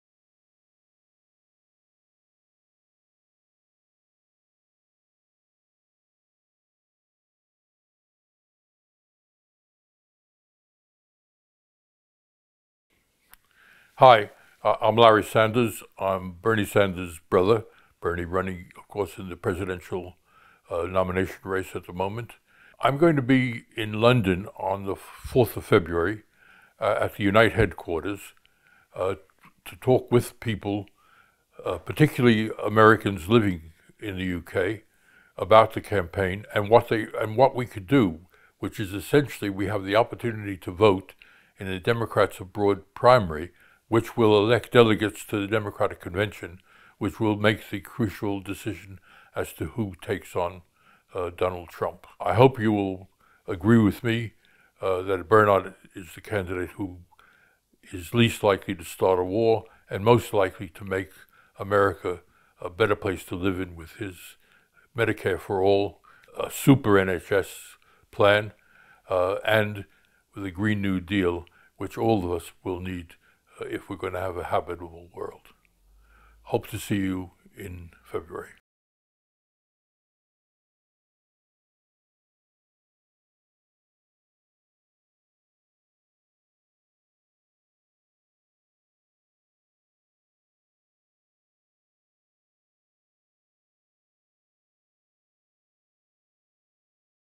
Larry Clip 6 London
Larry Sanders (brother of Bernie sanders) will be promoting the Bernie for Democrats Abroad 2020 at the Unite HQ, London on Feb 4th, along with Paul Mason and other guests...